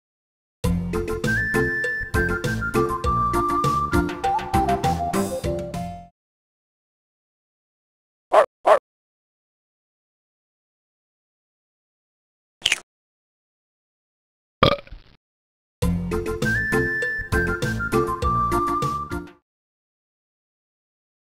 Pez animation
A Pez commercial I made for my Advertising class for fun. Brings in the idea of what it would be like if we, ourselves, were a PEZ dispenser.